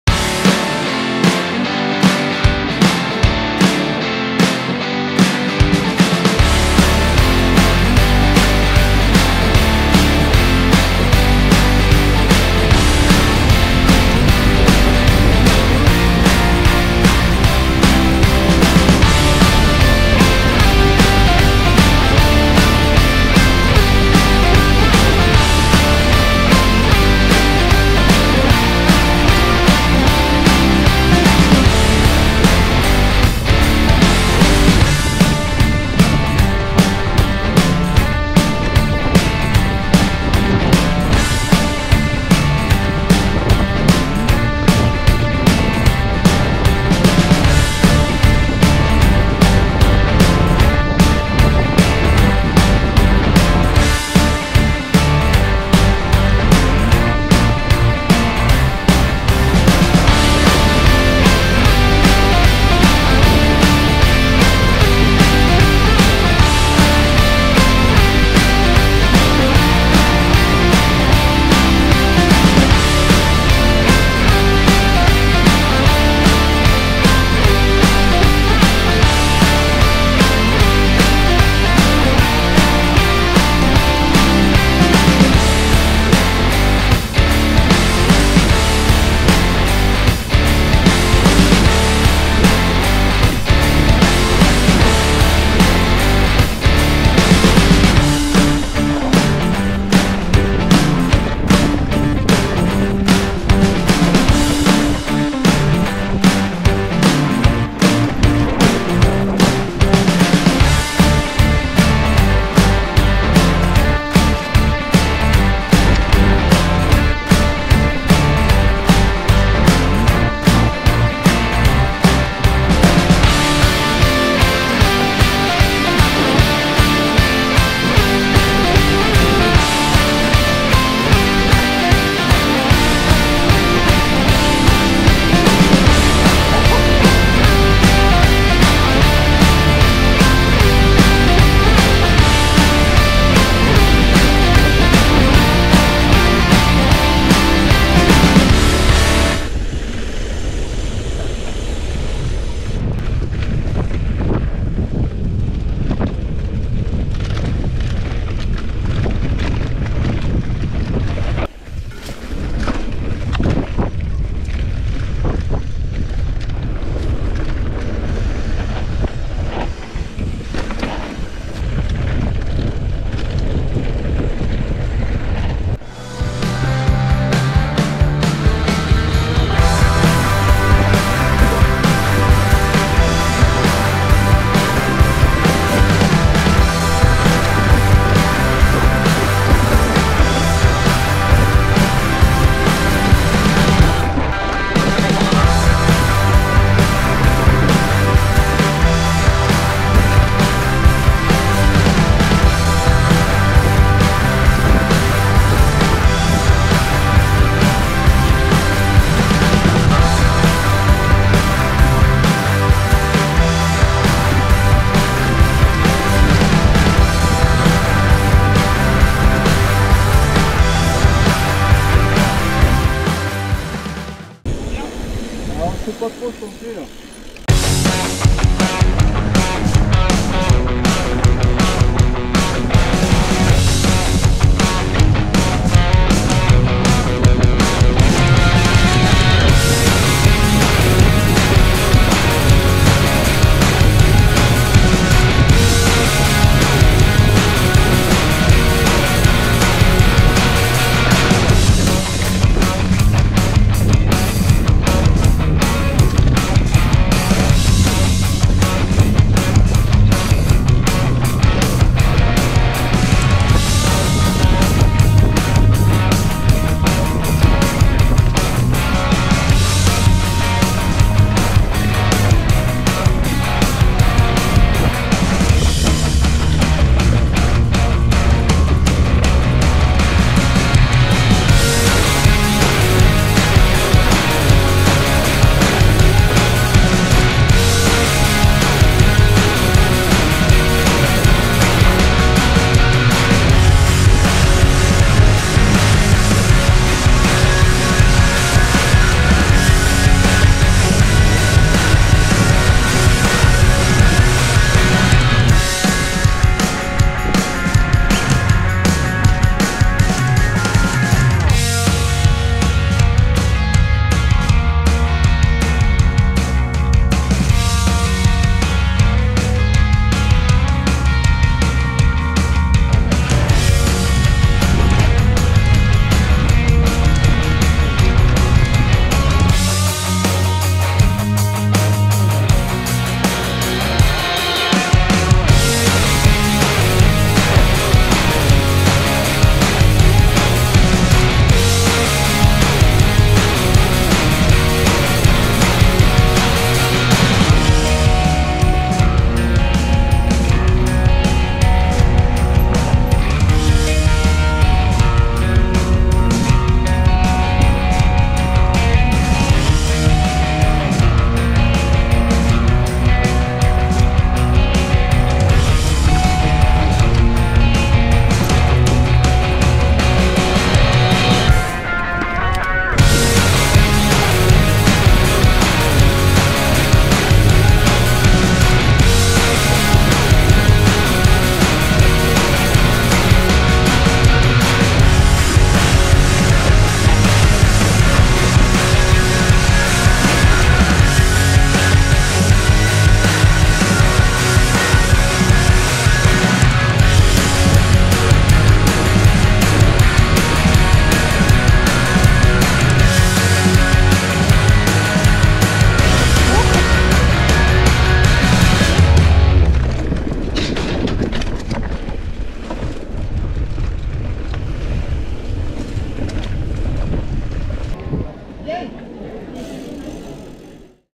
VTT Rando Aude : Les Coustous 2022 (Villasavary)
Parcours de VTT filmé avec une Go Pro Hero 10. Dans l'Aude, à Villasavary entre Castelnaudary et Carcassonne à l'occasion de la 16ème randonnée "Les Coustous" organisée par le club VTT Villasavary : Lous Pounpils. Au programme randonnée VTT 30km/45km. Au guidon d'un Santa Cruz Nomad.